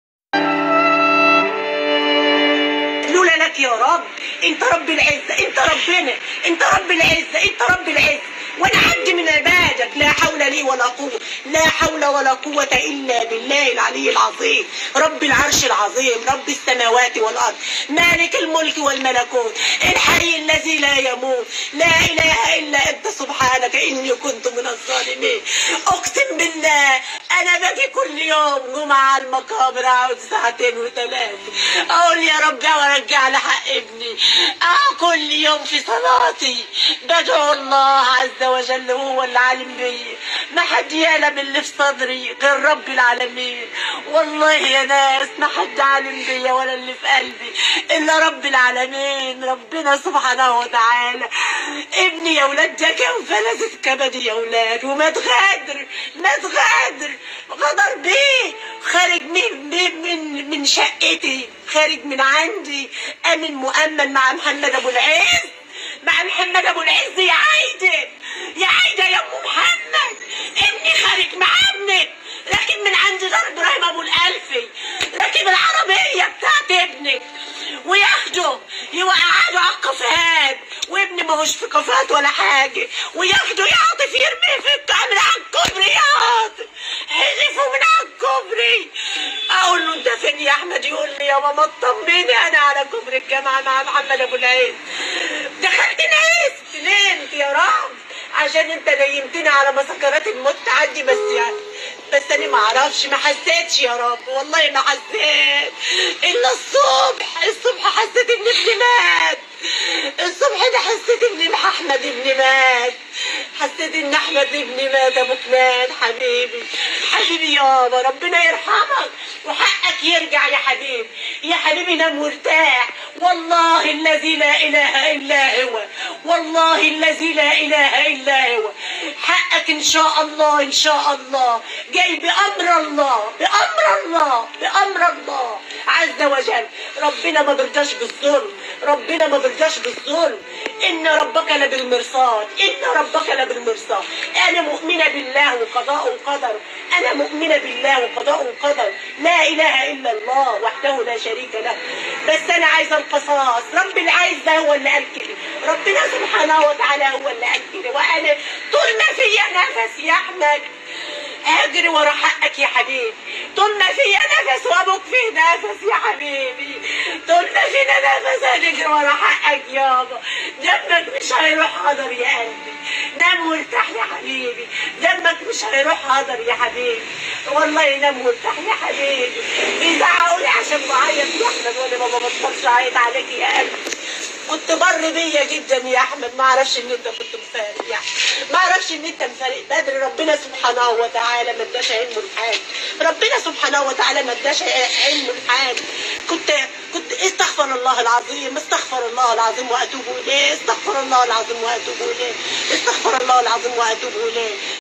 انهيار والدة مهندس الدقهلية بعد احالة المتهم للجنايات # غدر الصحاب
انهيار والدة مهندس الدقهلية عند قبره بعد احالة المتهم للجنايات